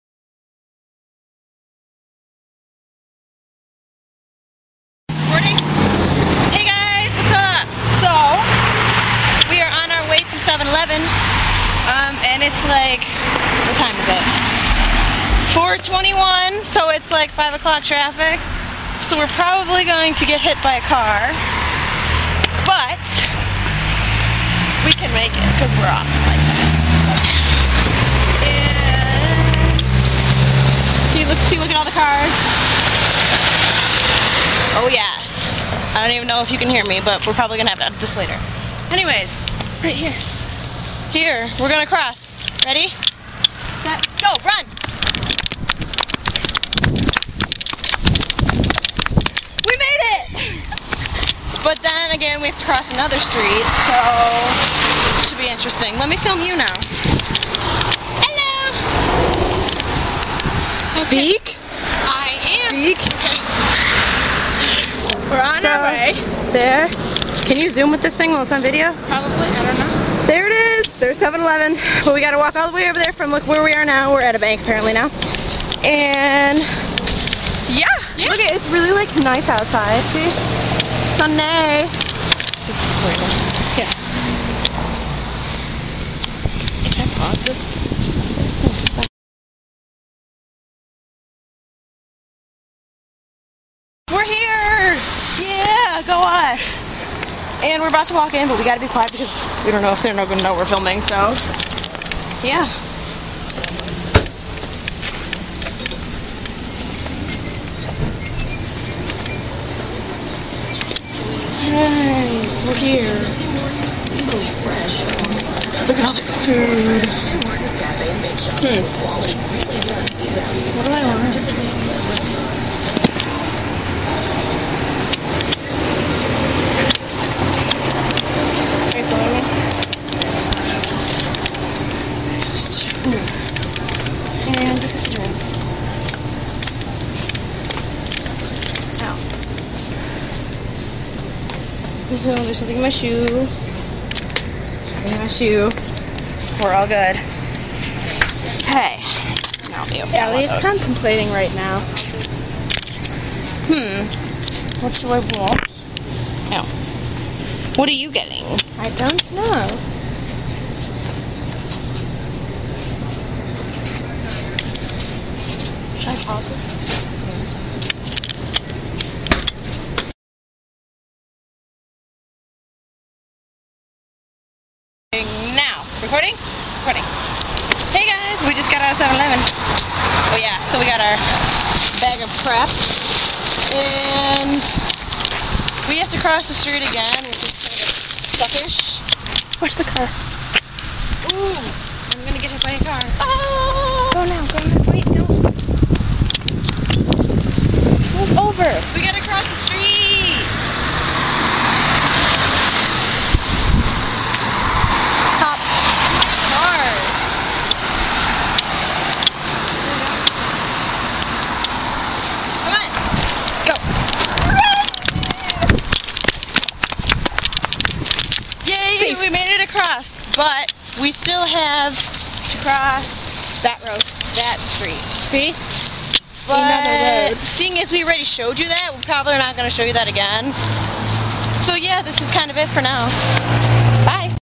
7-11 adventures!!
me and Jess, walking to 7-11, enjoy!!